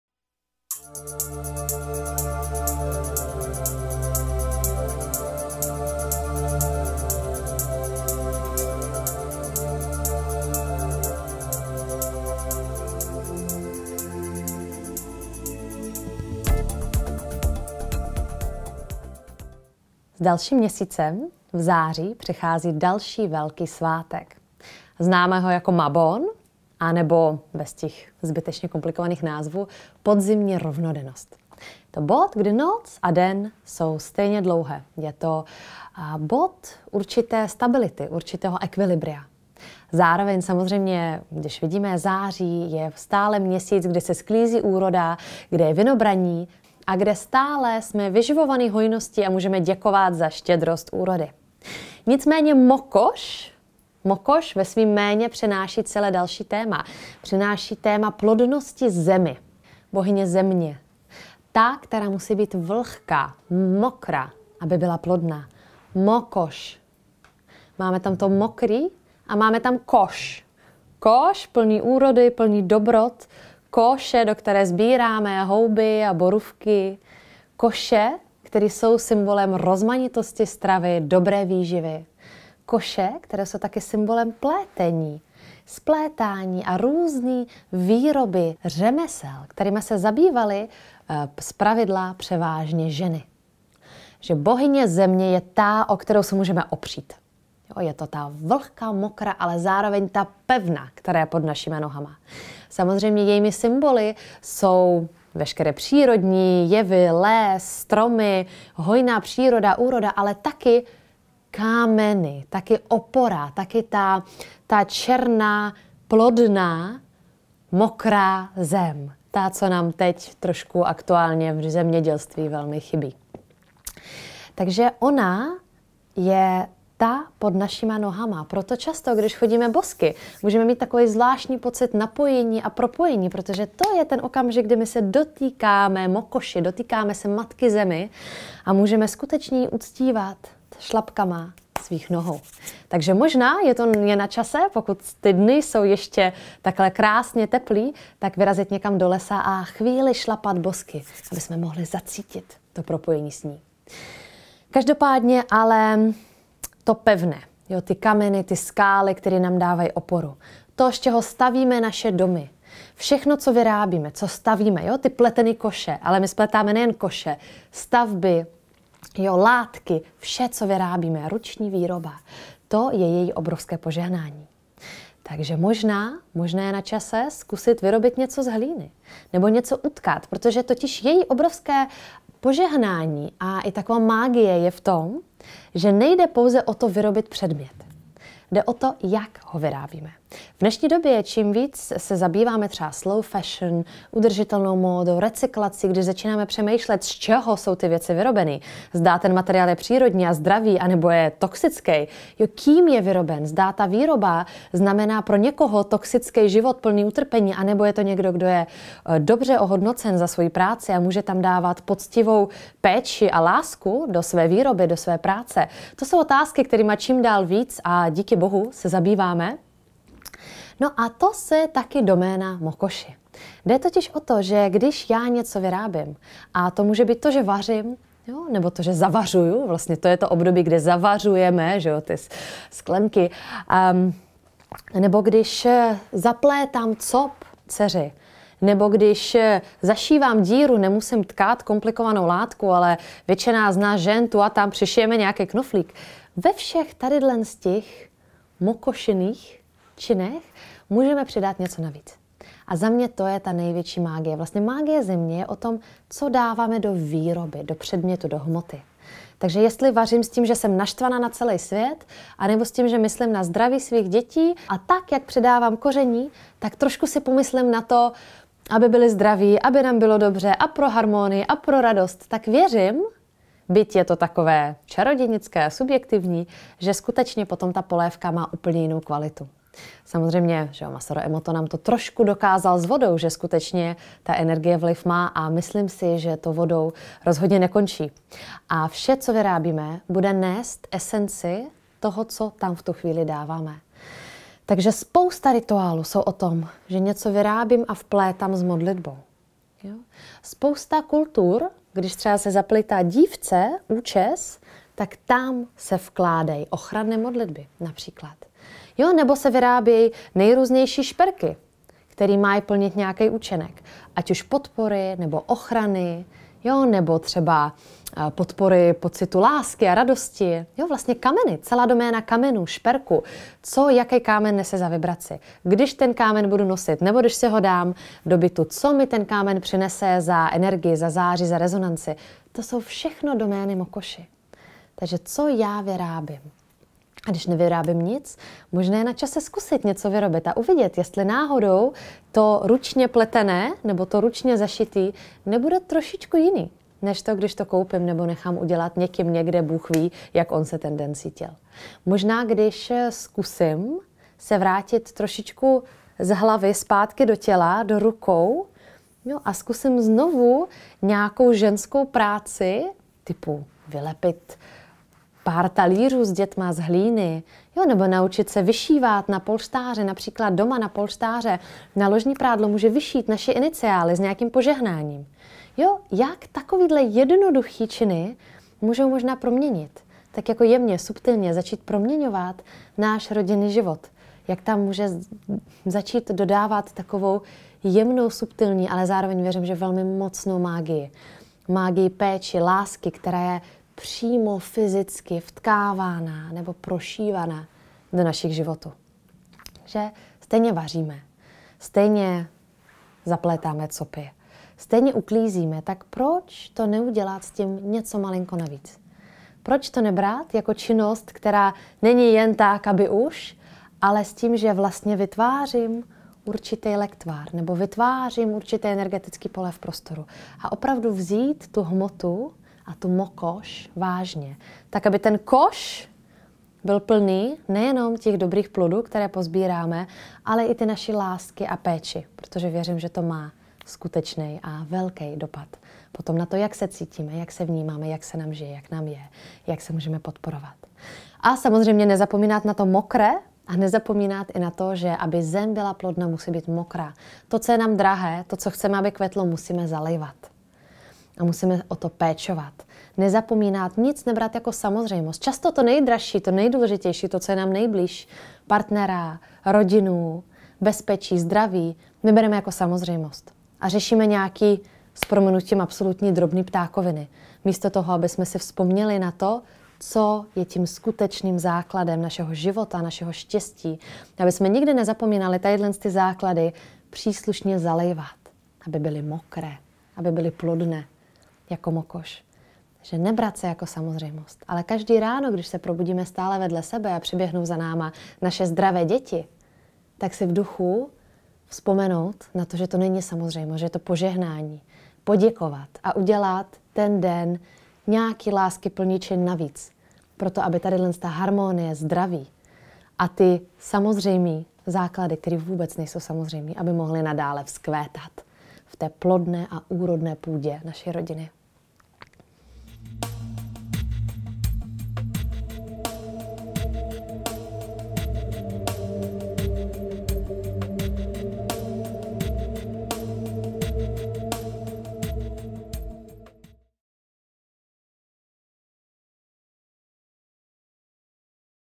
MOKOŠ: Bohyně úrodné půdy a patronka lidských osudů
Mokoš je bohyní země. Země, která musí být mokrá, vlhká, aby byla plodná. Jsme vyživováni její hojností a za štědrost úrody jí patří náš dík.
VÍCE O PROJEKTU Jsem žena jsembohyne